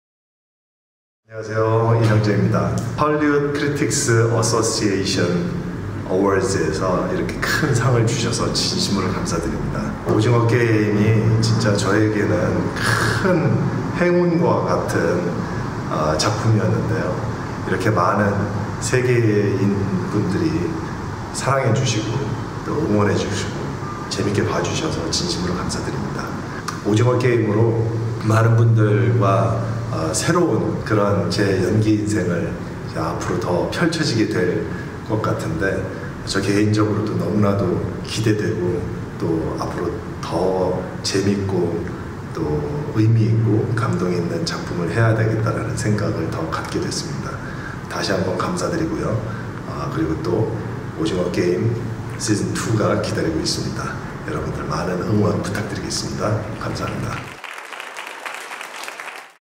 Lee Jung-Jae Speech | 2022 HCA TV Awards | Netflix’s Squid Game
SquidGame star Lee Jung-Jae wins for Best Actor in a Streaming Series, Drama at the 2nd Annual Hollywood Critics Association Television Awards.

Lauren Huff + Nestor Bentancor presented the award to LeeJungJae

The HCA TV Awards show are streaming live from The Beverly Hilton Hotel.

Twitter: @HCACritics - Instagram/Facebook: @HollywoodCriticsAssociatio